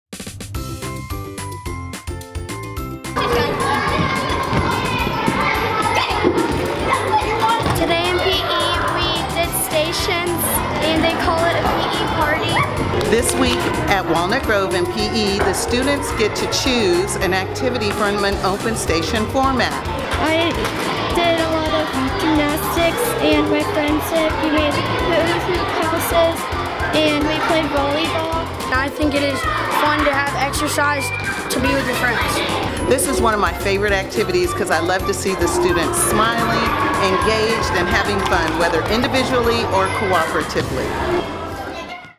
Walnut Grove students enjoy a variety of activities in PE
In this School Snapshot, students at Walnut Grove have fun as they get to choose from a wide array of activities in PE class. Thank you to WGES Good News Ambassador Dwayne Hastings for providing this footage.